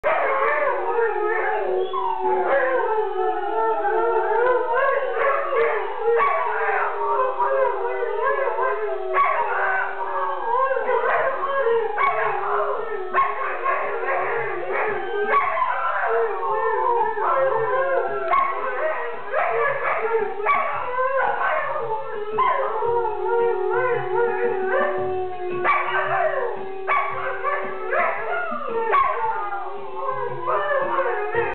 los galgos tambien cantan!
cantando un tema de lagwagon